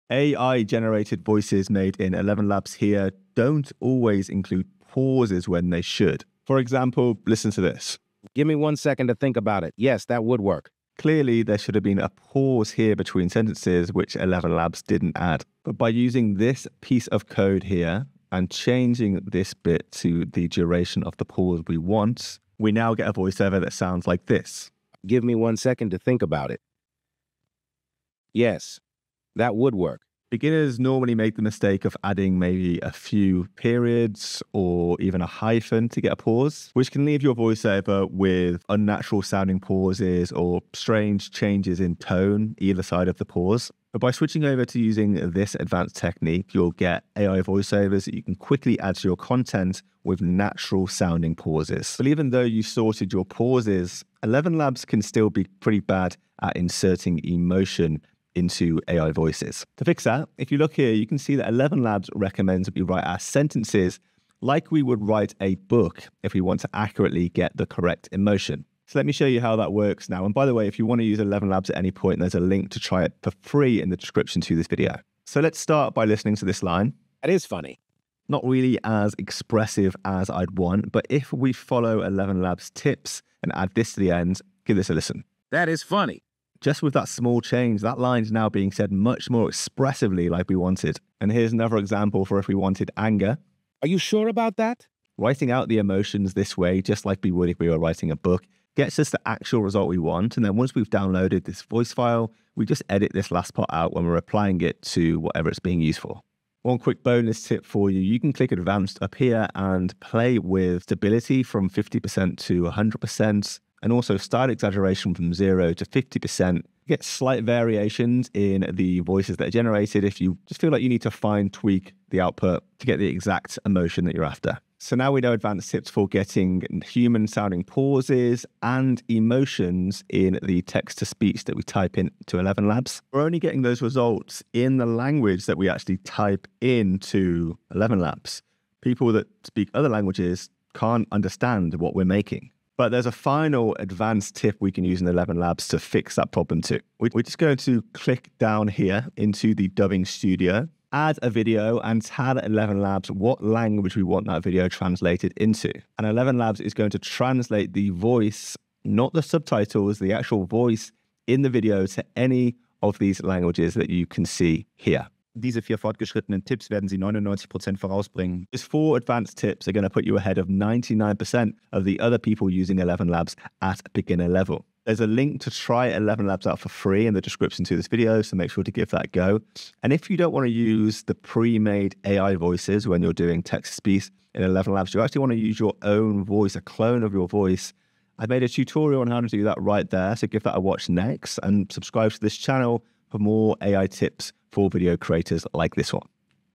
4 Elevenlabs Voice Tips in 3 Minutes (add pauses, emotions & dubbing)
4 advanced ElevenLabs voice tips in 3 minutes to put you ahead of 99% of the people who use it it at beginner level.

These are the best ElevenLabs voice settings to add pauses, emotions, and dubbing!

✅ Poppy AI for Research (GREGP = $25 off)

00:00 Add pauses
00:52 Add emotions
01:45 Bonus Tip
02:04 Dubbing